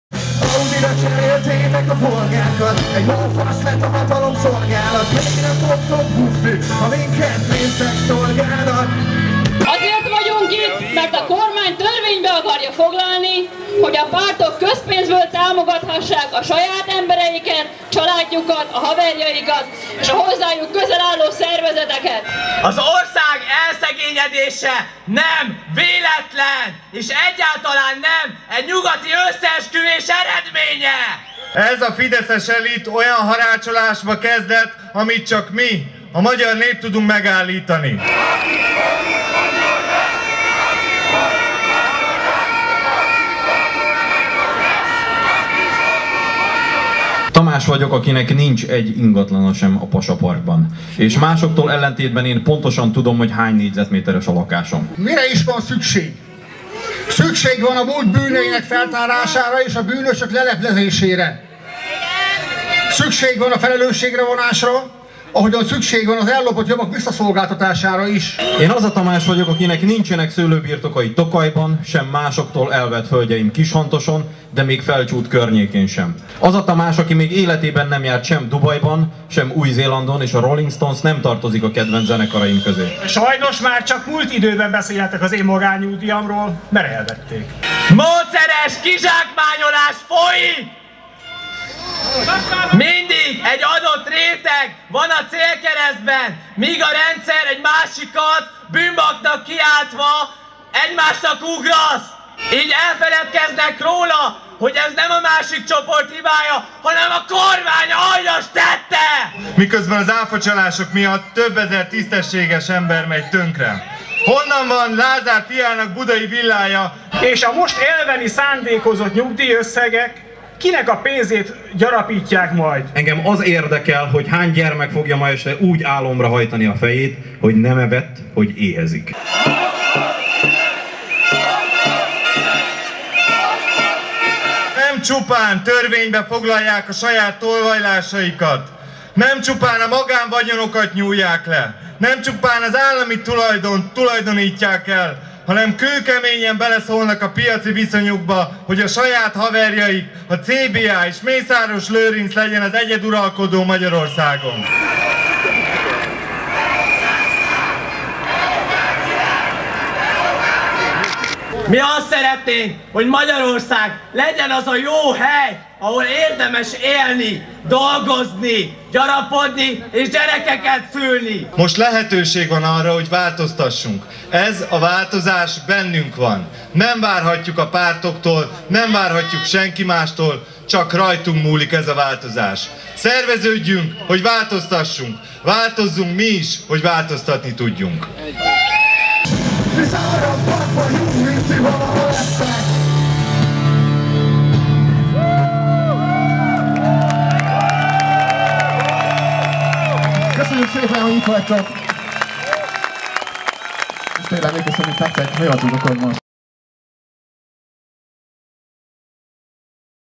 Ma mutyi, holnap dutyi
Ne várjunk másokra, vessünk véget mi a korrupciónak - üzenték több ezren a csütörtöki tüntetésen